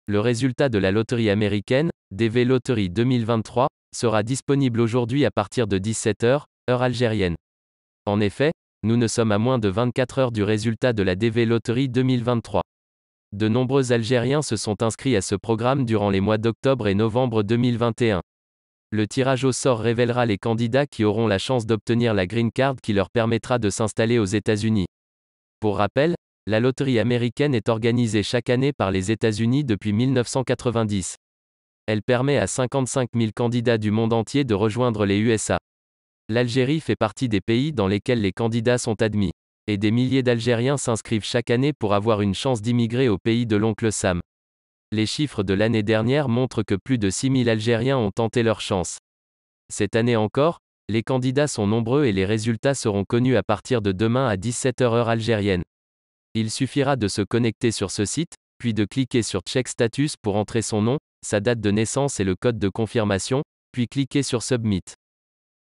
DV Lottery 2023 : lien pour consulter les résultats de la loterie américaine
Nous sommes à moins de 24 heures des résultats de la loterie américaine, DV Lottery 2023 qui seront disponible aujourd'hui à partir de 17h00, GMT+1 (heure algérienne). En effet, nombreux Algériens se sont inscrits à ce programme durant les mois d’octobre et novembre 2021. Ainsi, le tirage au sort révélera les candidats qui auront la chance d’obtenir la Green Card qui leur permettra de s’installer aux États-Unis. Par ailleurs, la loterie américaine est organisée chaque année par les États-Unis depuis 1990 et permet à 55000 candidats du monde entier de rejoindre les USA. Effectivement, l’Algérie fait partie des pays dans lesquels les candidats sont admis, ainsi, des milliers d’Algériens s’inscrivent chaque année pour avoir une chance d’immigrer au pays de l’Oncle Sam.